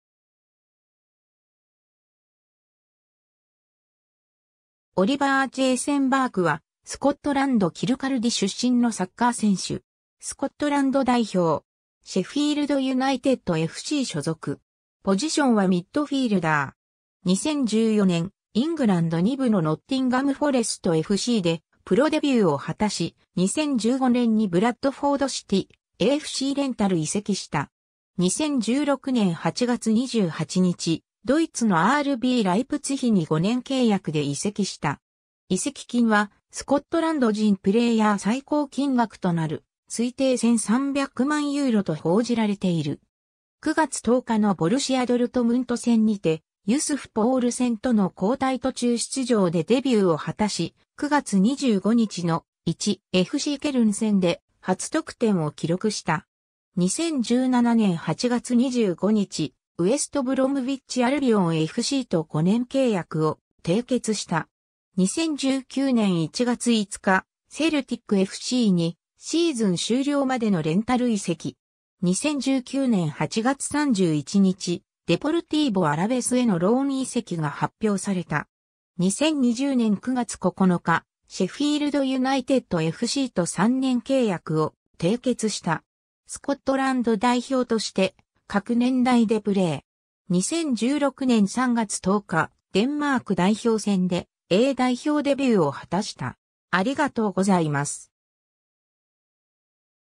オリヴァー・バーク
ブラッドフォード・シティAFCの選手
デポルティーボ・アラベスの選手
シェフィールド・ユナイテッドFCの選手
在スペイン・イギリス人
オリヴァー・ジェイセン・バーク（Oliver Jasen Burke, 1997年4月7日 - ）は、スコットランド・キルカルディ出身のサッカー選手。
スコットランド代表。
シェフィールド・ユナイテッドFC所属。
 2014年、イングランド2部のノッティンガム・フォレストFCでプロデビューを果たし、2015年にブラッドフォード・シティAFCレンタル移籍した。
 2016年8月28日、ドイツのRBライプツィヒに5年契約で移籍した。
移籍金はスコットランド人プレイヤー最高金額となる推定1300万ユーロ（約15億円）と報じられている 。
9月10日のボルシア・ドルトムント戦にてユスフ・ポウルセンとの交代途中出場でデビューを果たし、9月25日の1.FCケルン戦で初得点を記録した。
 2017年8月25日、ウェスト・ブロムウィッチ・アルビオンFCと5年契約を締結した。
 2019年1月5日、セルティックFCにシーズン終了までのレンタル移籍。
 2019年8月31日、デポルティーボ・アラベスへのローン移籍が発表された。
 2020年9月9日、シェフィールド・ユナイテッドFCと3年契約を締結した。
 スコットランド代表として各年代でプレー。
2016年3月10日、デンマーク代表戦でA代表デビューを果たした。